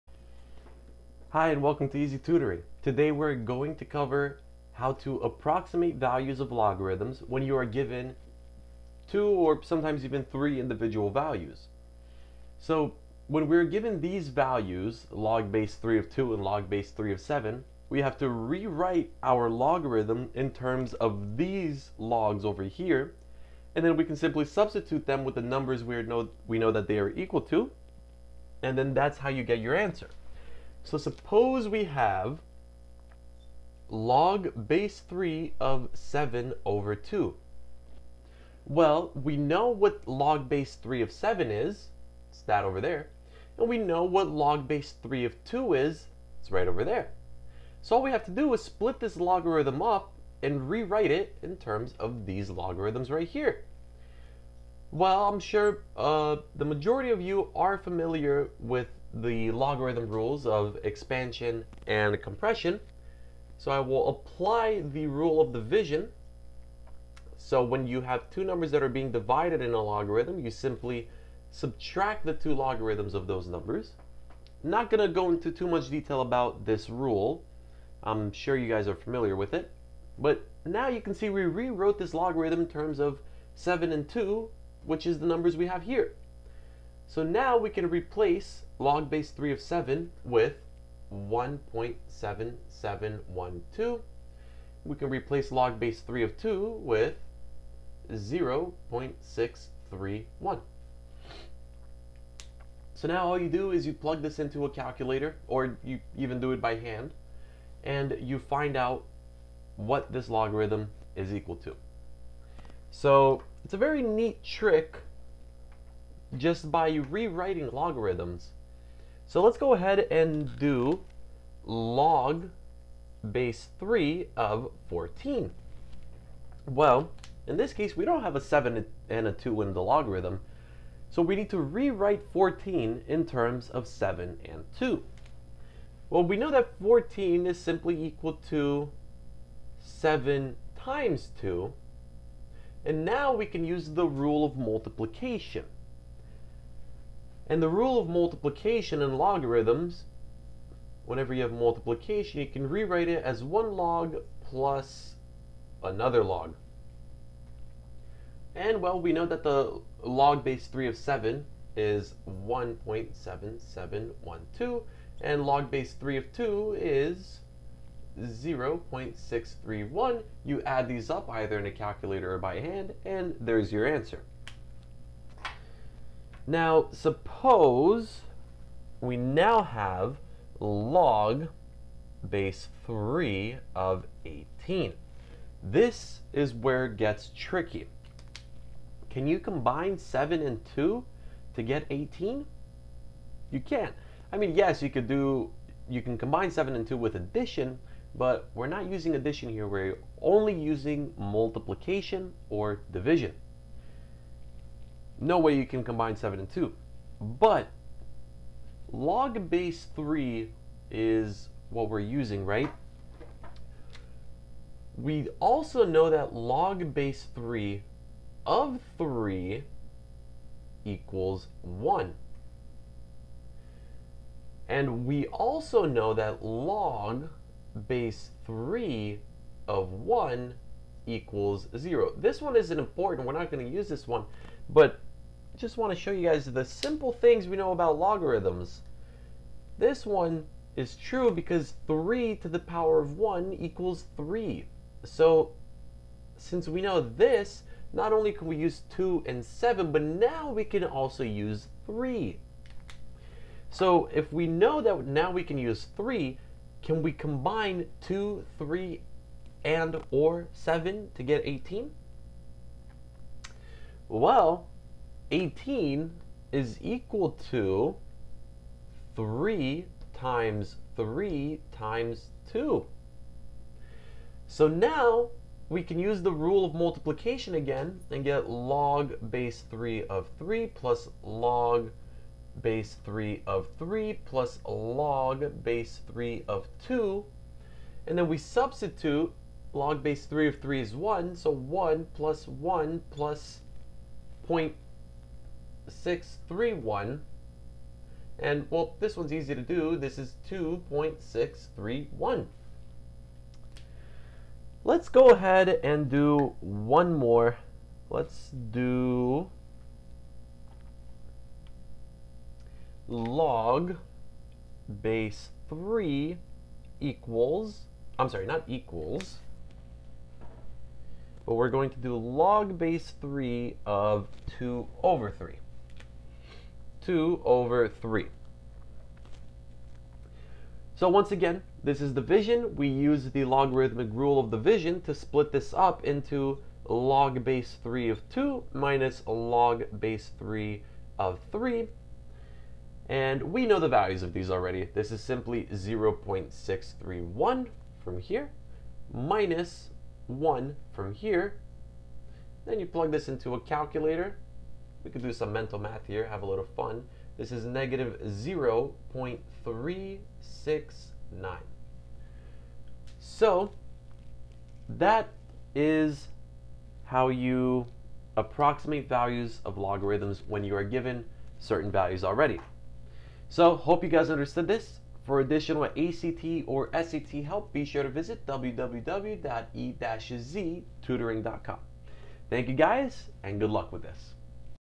Approximate Values of Logarithms - Algebra 2 - E-Z Tutoring
In this video we dive into approximating values of logarithms.